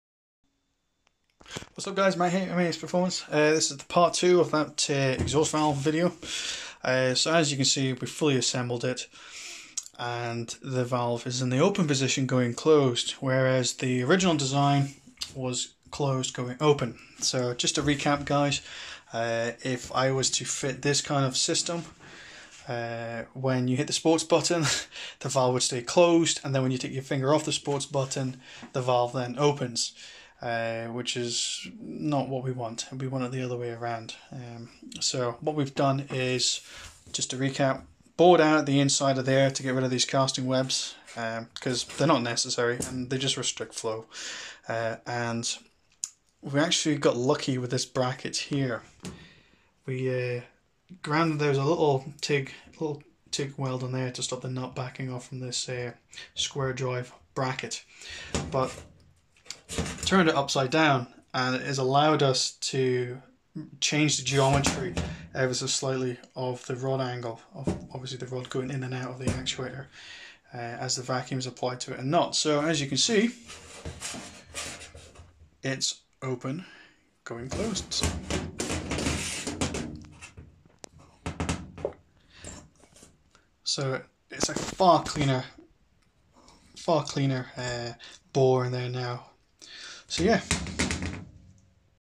MAS Performance exhaust valve modifications part 2
In light of keeping my neighbours happy and less drone on the motorway I've decided to fit a aftermarket exhaust valve to my custom system.
The 8P RS3 has a sports button which one of its benefits is opening the factory exhaust valve which at the time I didn't think I'd bother getting incorporated into my custom system and now I'm wishing I had. The problem with most of these aftermarket exhaust valves is they're closed going open where as I need open going closed as the factory exhaust valve works via vacuum so has required a fair bit of scratching the head